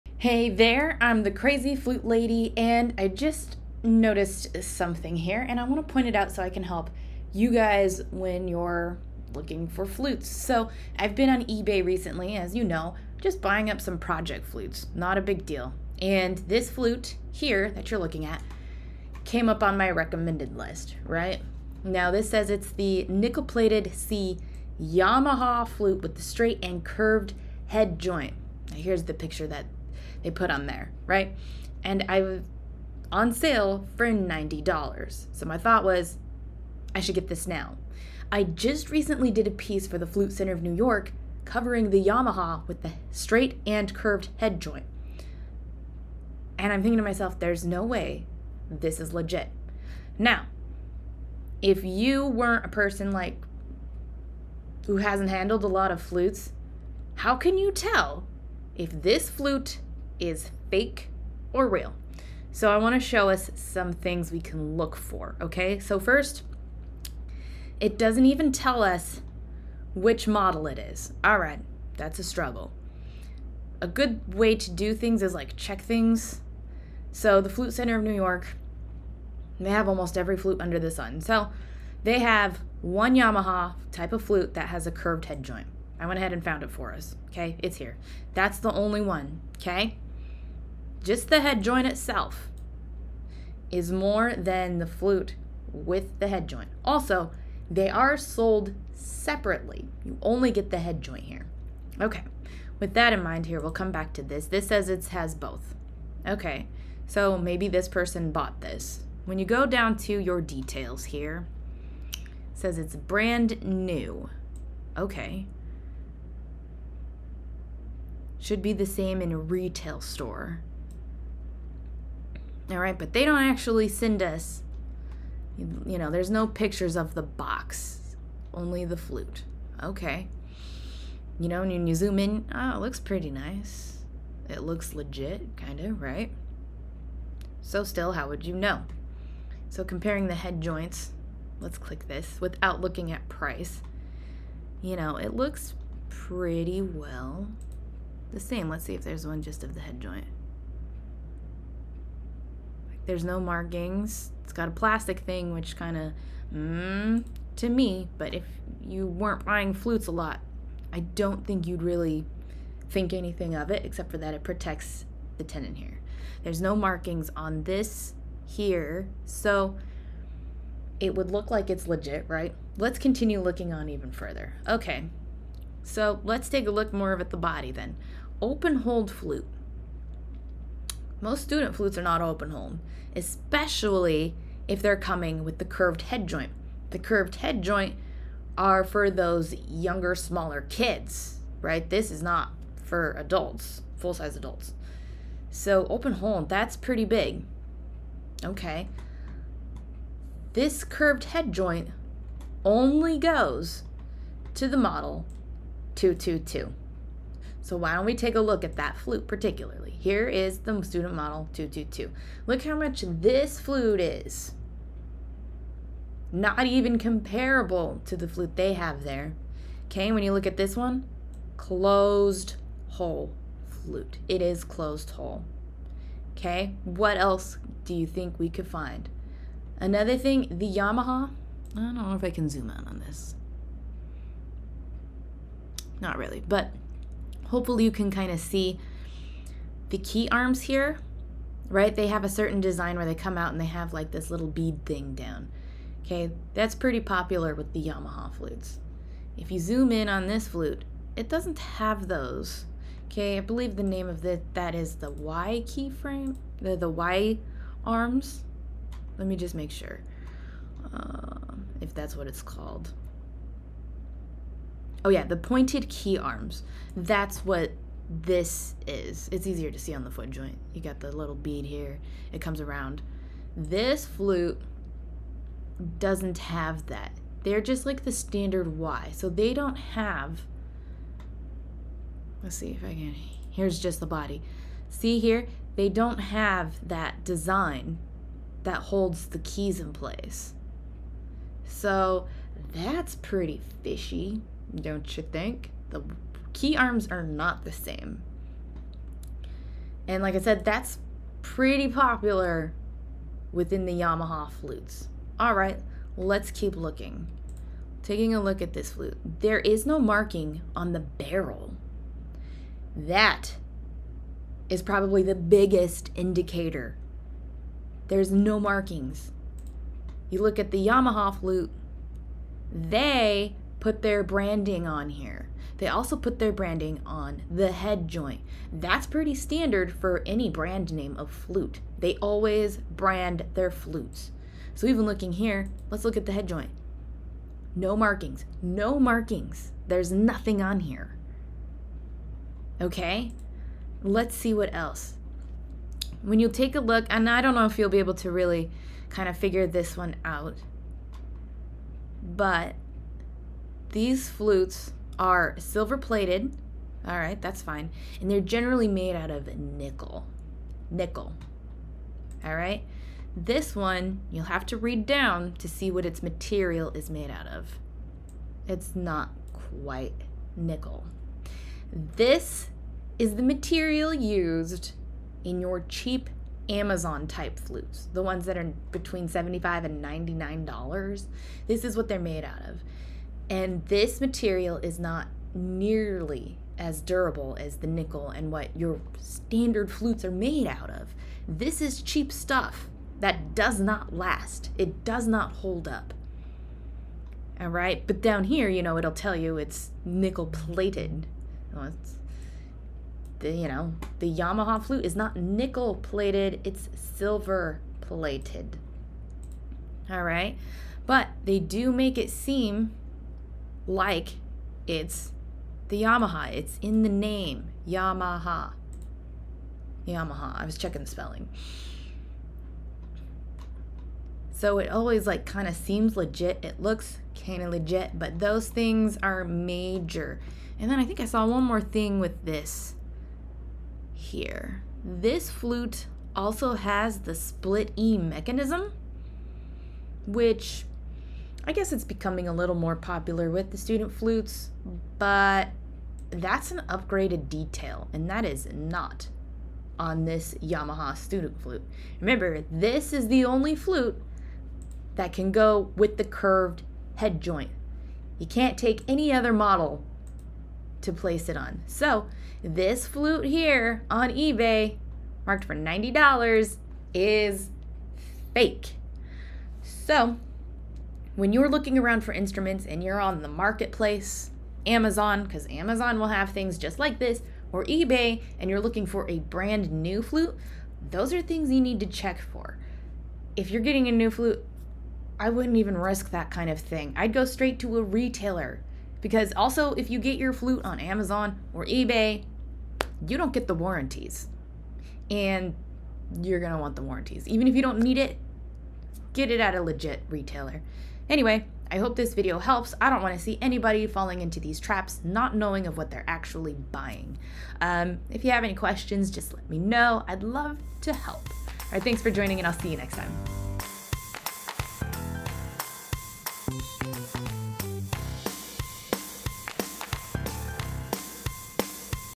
Is This Yamaha Flute Real Or Fake? How Can You Tell?
Hey there! It's the Crazy Flute Lady! I've been buying project flutes off of Ebay recently and a suggested item was a Yamaha flute with both the straight and curved headjoint for a hugely discounted price. Is this Yamaha flute real or fake? How you can tell? I have a pretty good idea, and just wanted to share my findings with you so you can be more mindful when flute shopping.

If you are shopping for a flute, go to a real retailer. My favorite flute shops are Flutistry and the Flute Center Of New York. They'll take good care of you.